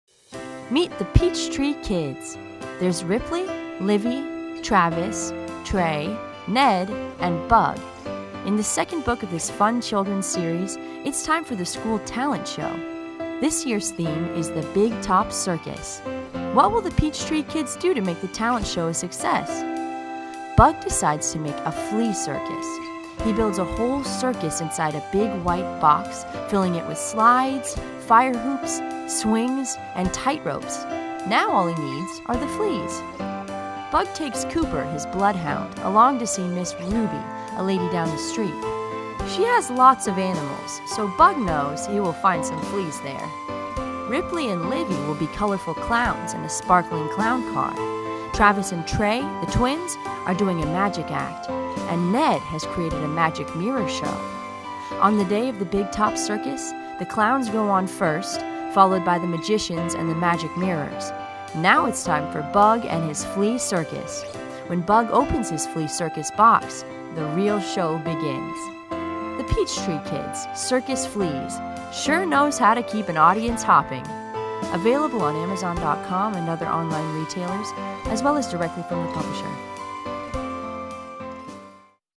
"The Peachtree Kids, Circus Fleas" by Jennie Chadwick
Jennie Chadwick  /  The Peach Tree Kids: Circus Fleas    Eloquent Books  /  Softcover  /  5x8  /  96 pages  /  $9.99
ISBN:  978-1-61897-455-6

Meet The Peach Tree Kids. There's Ripley, Livy, Travis, Tray, Ned and Bug. In the second book of this fun children's series, it's time for the school talent show. This year's theme is the Big Top Circus. What will The Peach Tree Kids do to make the talent show a success?
 Bug decides to make a Flea Circus. He builds a whole circus inside a big white box, filling it with slides, fire hoops, swings and tightropes. Now all he needs are the fleas!
 Bug takes Cooper, his bloodhound, along to see Miss Ruby, a lady down the street. She has lots of animals, so Bug knows he will find some fleas there.
 Ripley and Livy will be colorful clowns in a sparkling clown car. Travis and Tray, the twins, are doing a magic act, and Ned has created a magic mirror show.
 On the day of the Big Top Circus, the clowns go on first, followed by the magicians and the magic mirrors. Now it's time for Bug and his Flea Circus. When Bug opens his Flea Circus box, the real show begins!
 The Peach Tree Kids: Circus Fleas sure knows how to keep an audience hopping!
Jennie Chadwick lives and writes in the Central Valley of California, which is the backdrop for The Peach Tree Kids. She is currently at work on the next book in the series.